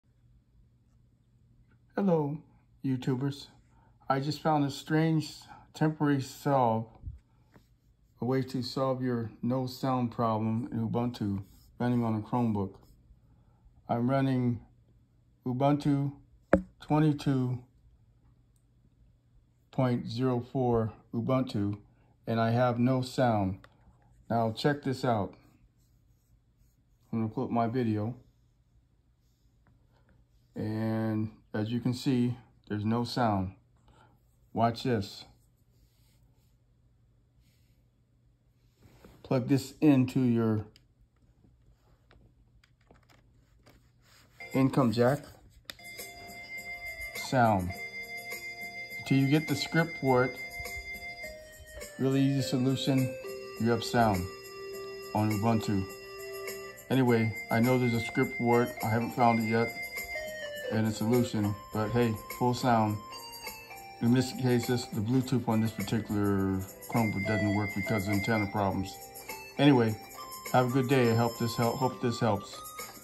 strange way to get sound working on Chromebook with Ubuntu
Correction Ubuntu 24.04